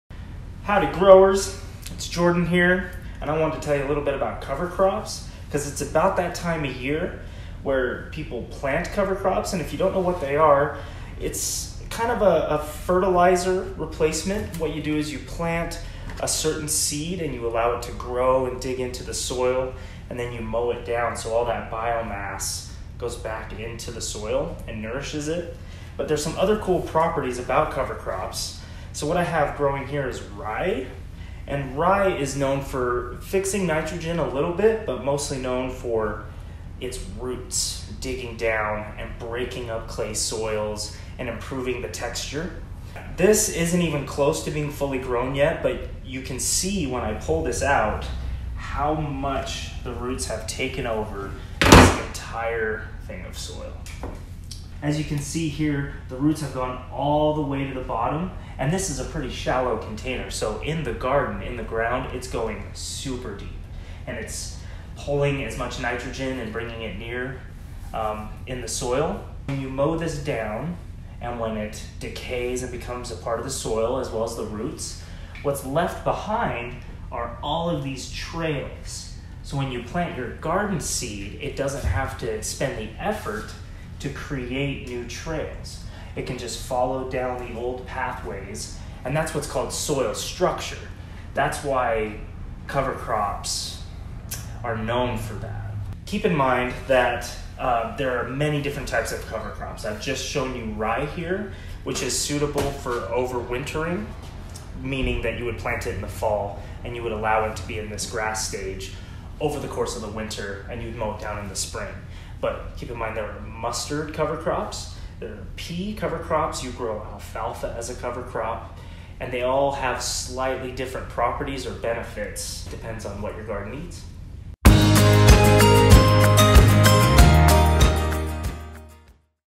Purpose of Cover Crops
Listen to Jordan explain the benefits of covercrops.  Here he has some rye grass.
Rye Grass-
Like many cereal grains, winter rye cover crop is popularly broadcasted directly in early spring, or early fall for second year maturity, intended to overwinter. For a traditional garden harvest, sow 2-3 heirloom winter rye cover crop seeds ½” deep and 2-3” apart in fertile, loamy, and well-drained garden soil in full sun. Non-GMO winter rye seeds germinate in 4-10 days, with no thinning required. Secale cereale is a winter-hardy grain with no serious pests or diseases. If sown to harvest rye grains and berries, the harvest winter rye when heads of seeds have become dried, hard, and heavy.

Depending on size of crop, anything from a pair of scissors to a scythe would work to cut down winter rye for drying. Using twine, bunch up loose rye into a bale, about the size of a bundle of wood. Allow winter rye cover crop to dry for 2-3 weeks before harvesting rye berries. Like many grains of the Triticeae family, heirloom winter rye berries can be milled, rolled, steel-cut, and boiled for nearly any occasion.